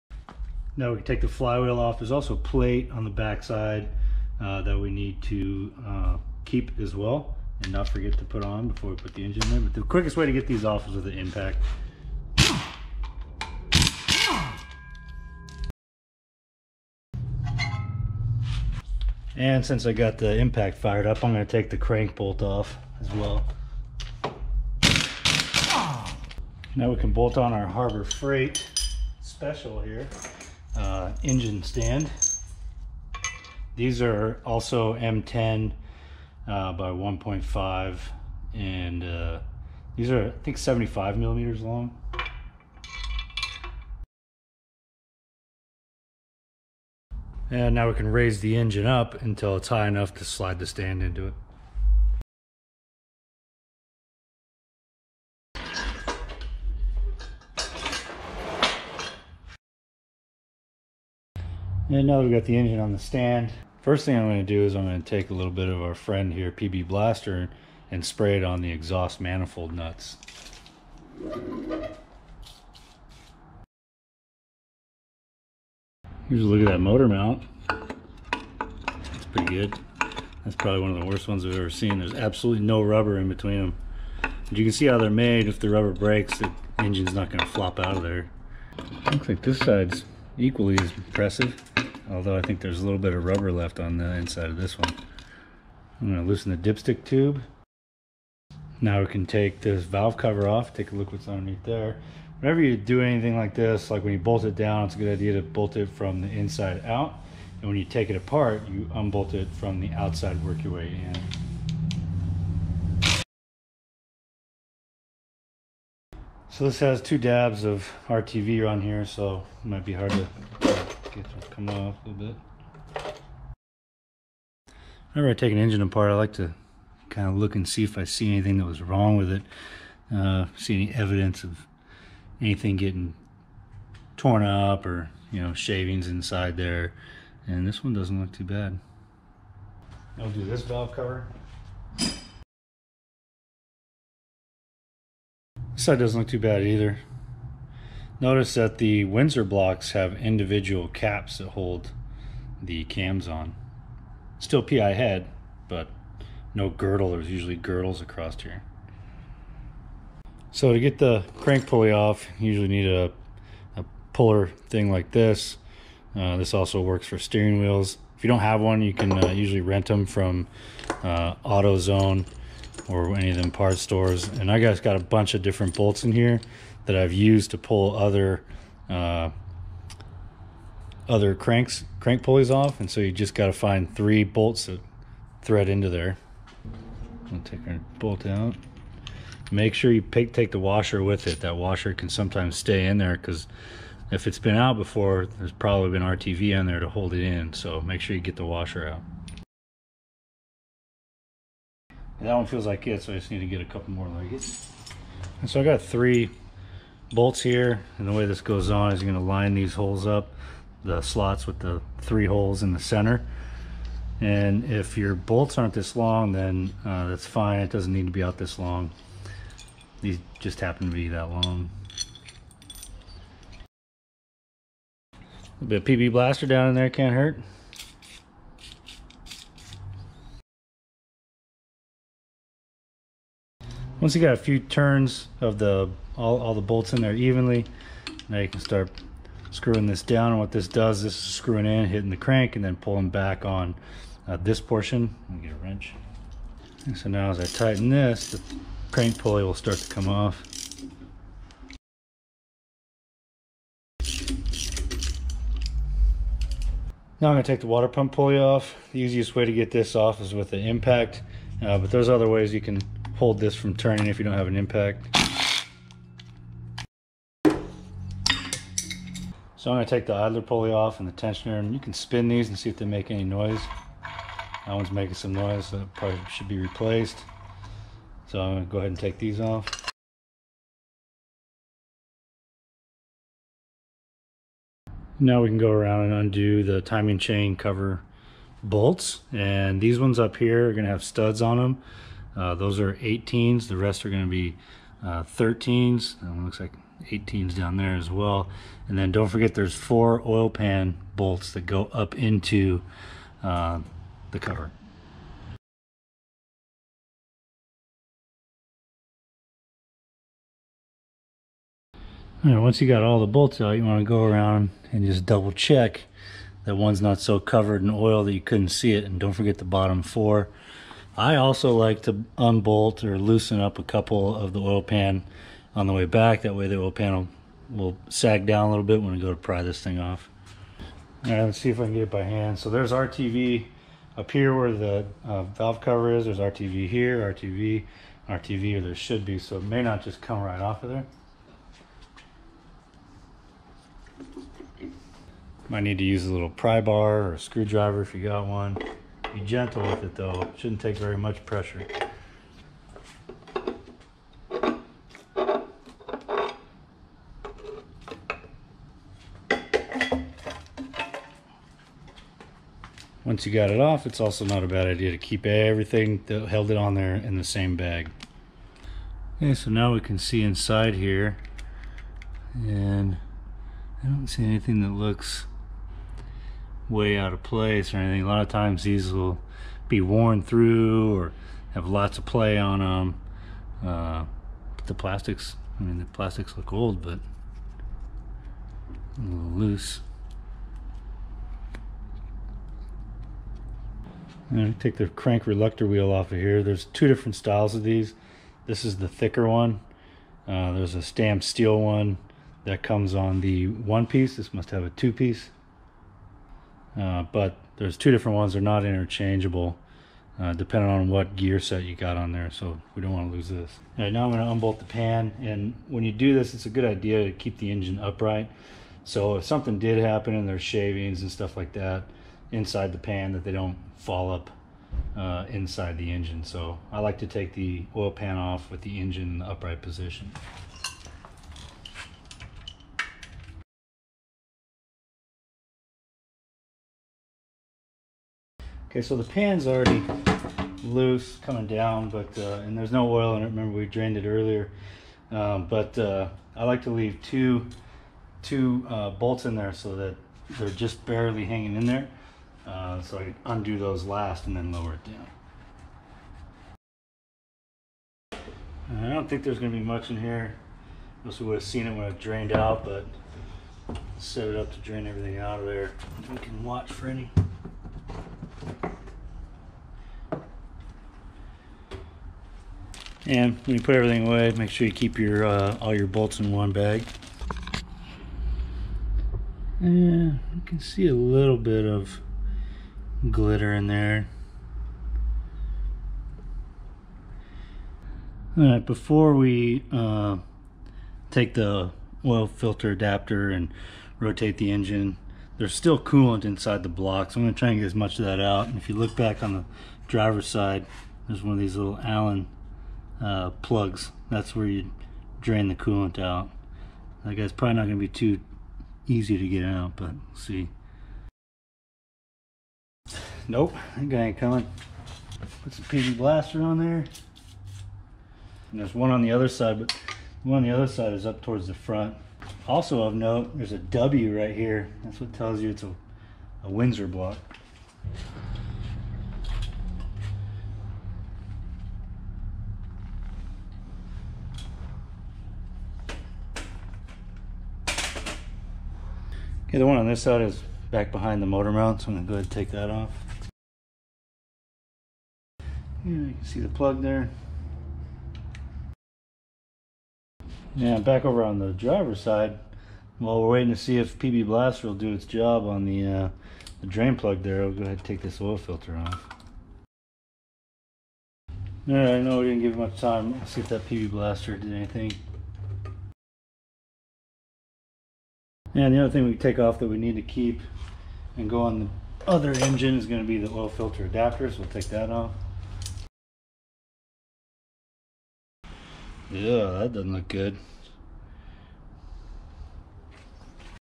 99 Mustang GT with Nemak Part 10 – Timing Chain & Valve Covers, Crank Pulley, and Oil Pan Removal
Continuing on the series, I remove more stuff from the long block to get what I need: the cylinder heads.  Let me know if you have any questions or comments below.